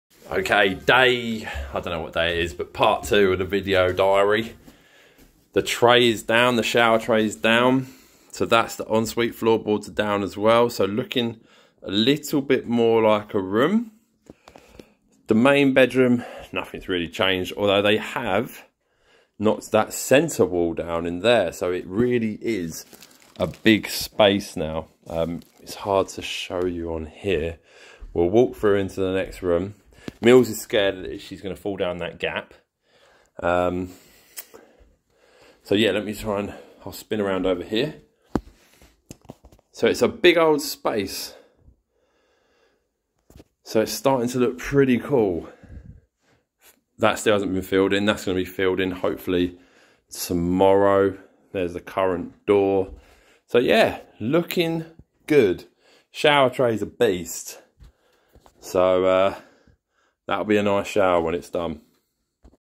Penthouse Build 'Part Two'
We are converting two bedrooms into one adding an ensuite and built in wardrobes and drawers :)